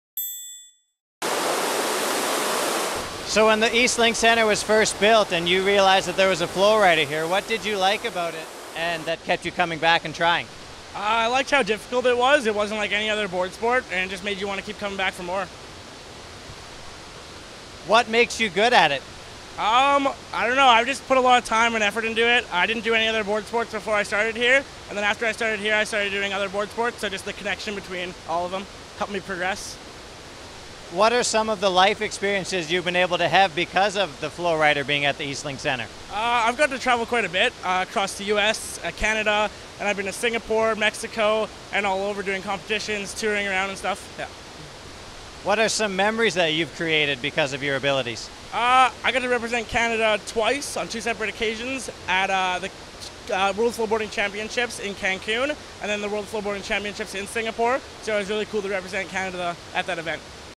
Eastlink Centre Staff Video: Kade
Eastlink Community TV in Grande Prairie produced a documentary about local people who are using the Eastlink Centre as a catalyst towards living healthy and active lives. This is a sample story from that documentary.